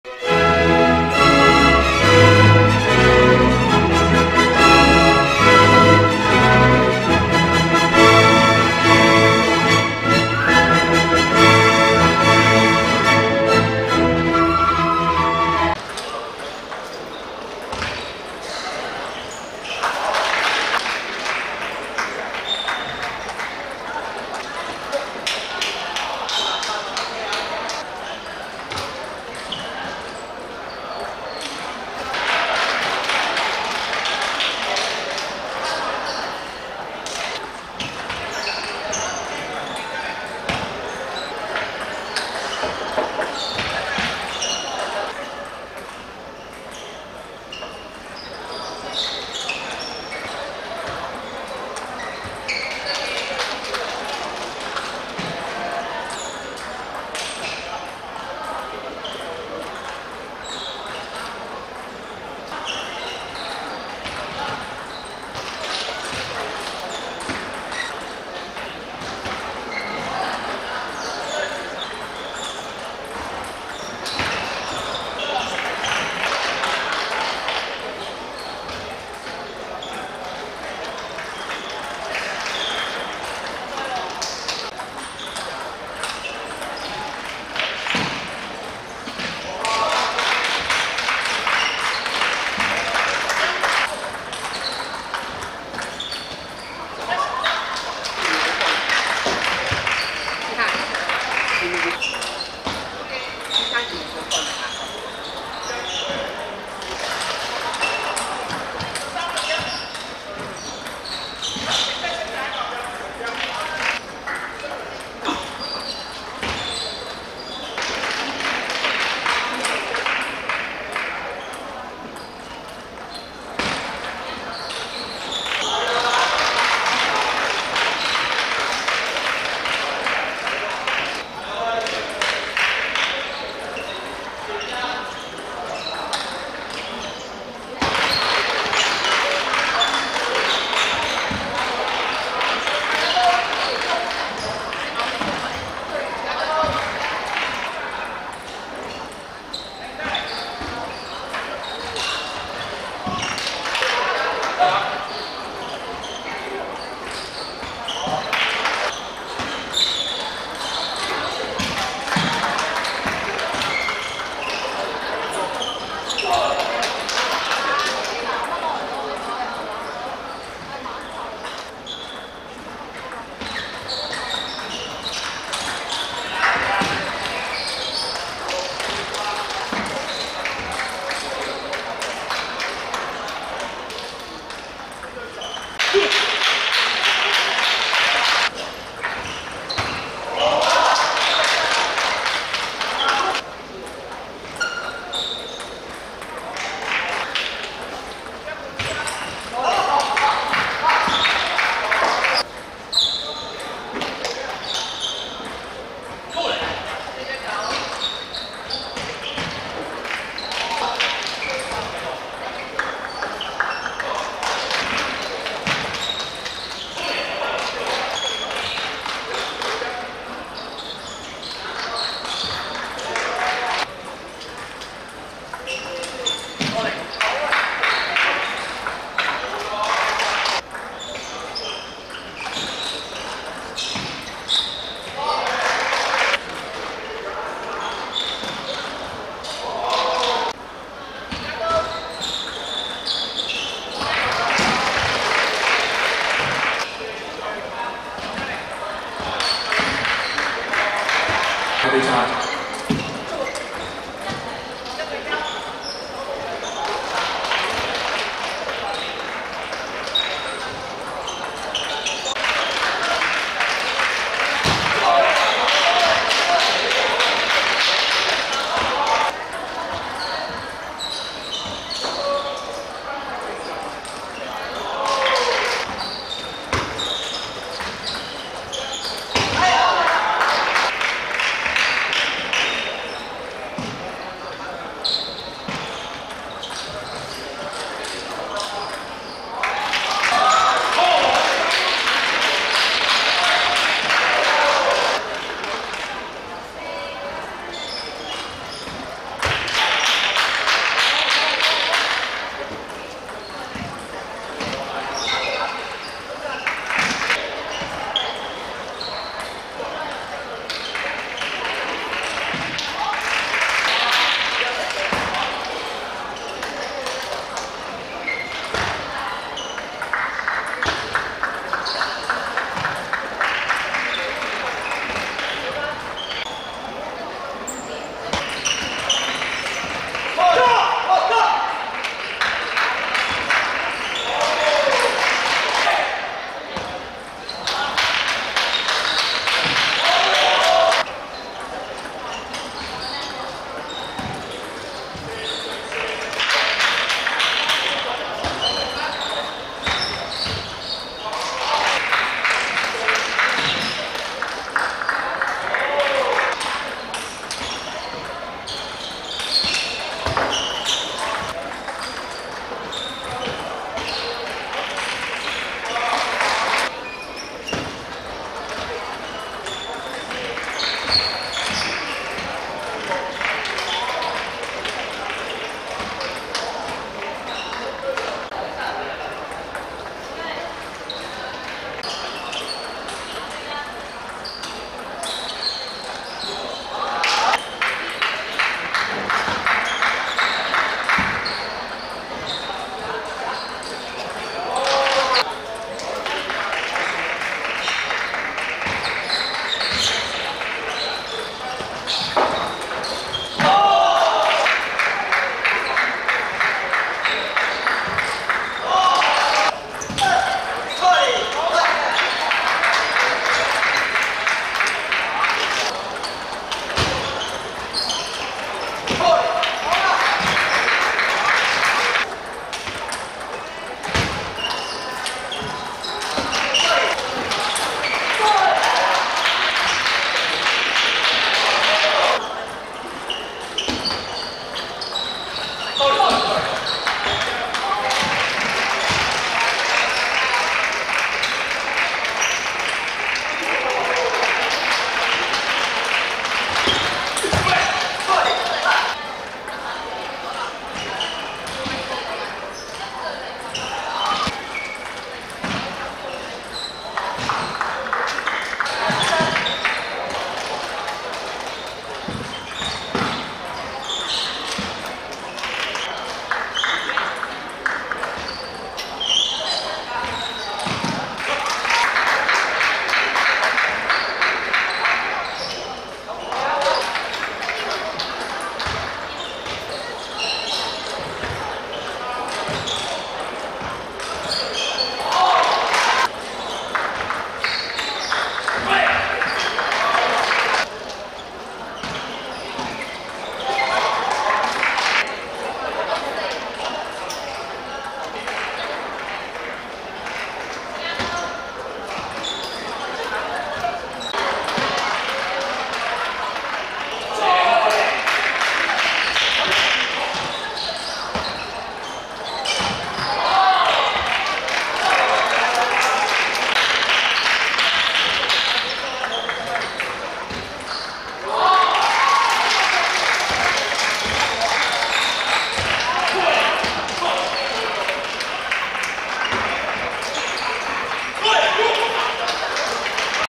東區vs元朗(2019.4.20.第七屆港運會女子乒乓球雙打三十二強)精華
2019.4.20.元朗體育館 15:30
東區(橙) 3 : 2 元朗(黑)
東區球員~ 何韻羚 林宛悠
元朗球員~史穎嘉  曾咏琪